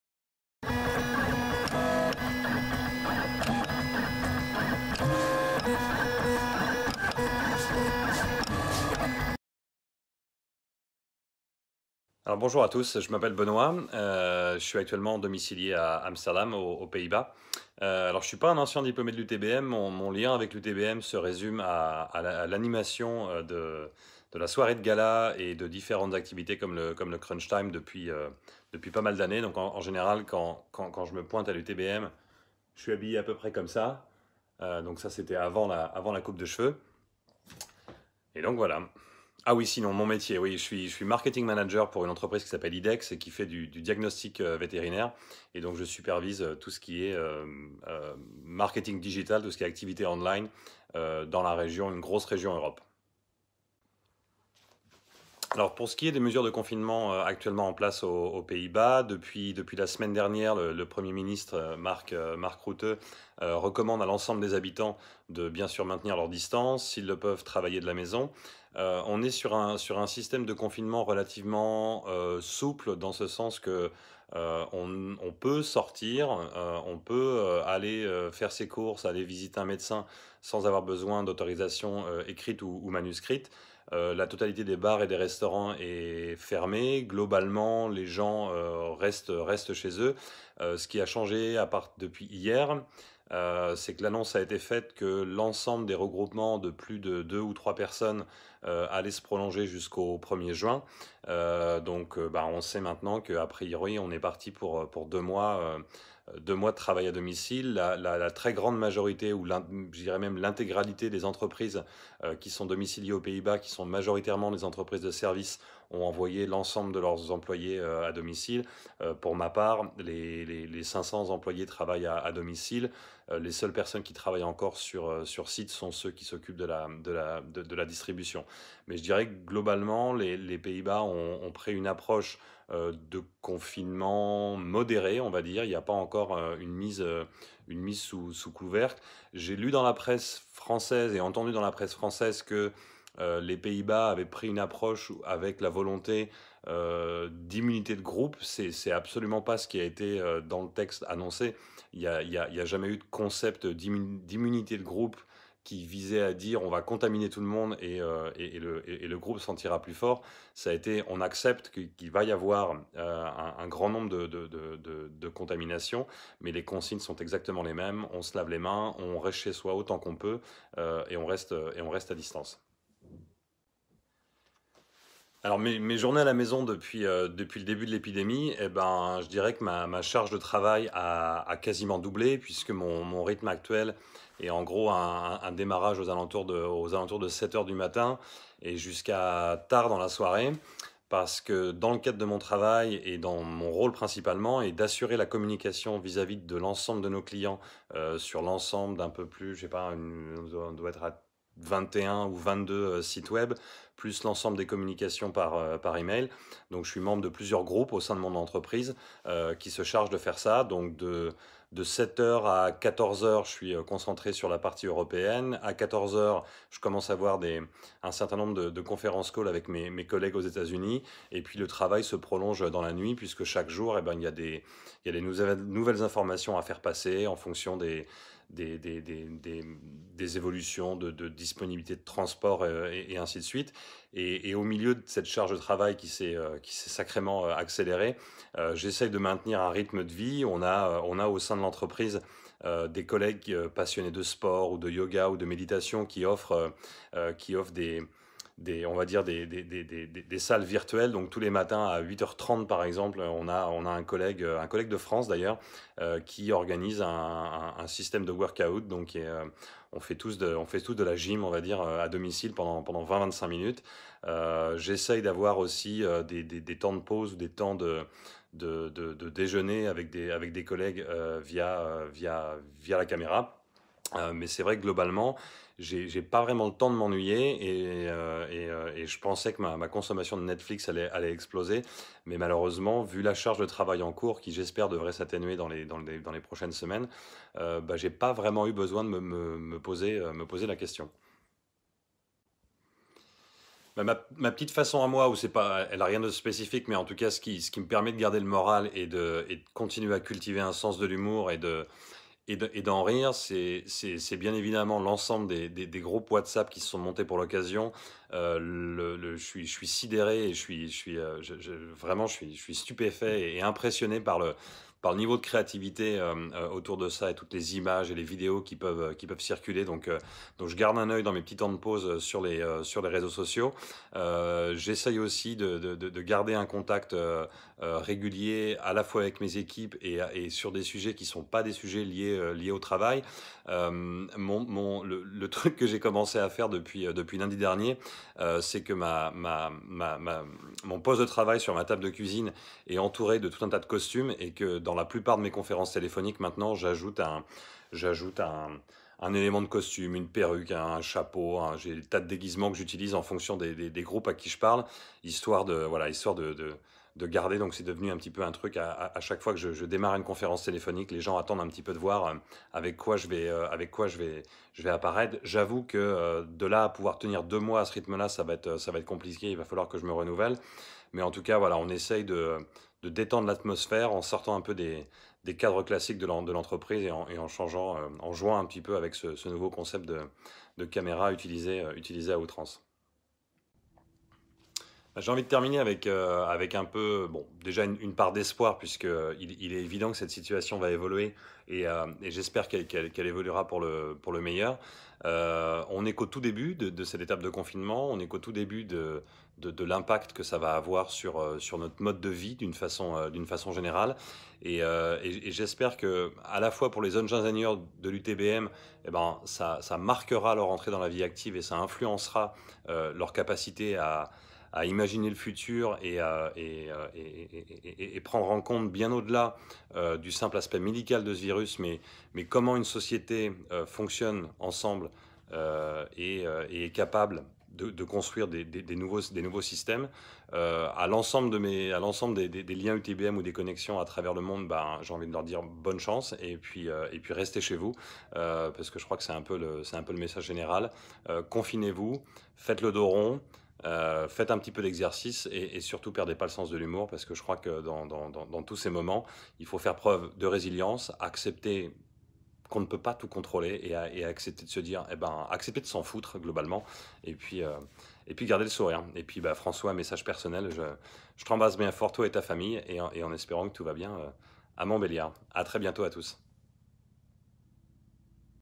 Benoit Leray - L'instant d'ailleurs #2
Temoignage de Benoit Leray - Présentateur officiel des cérémonies UTBM
Pays-bas

Partageons notre confinement ensemble !

Montage
Francois Jouffroy

Habillage Générique
Apollo77